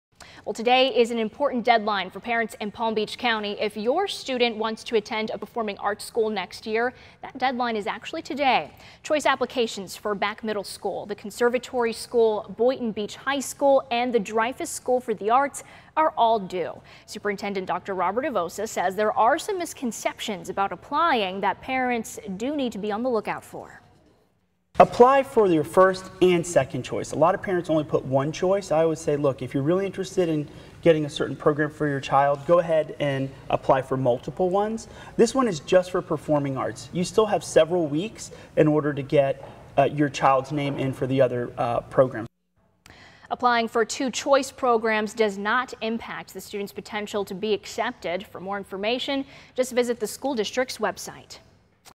Deadline to apply for performing arts school
Choice applications for Bak Middle School, The Conservatory School, Boynton Beach High School and the Dreyfoos for the Arts, are all due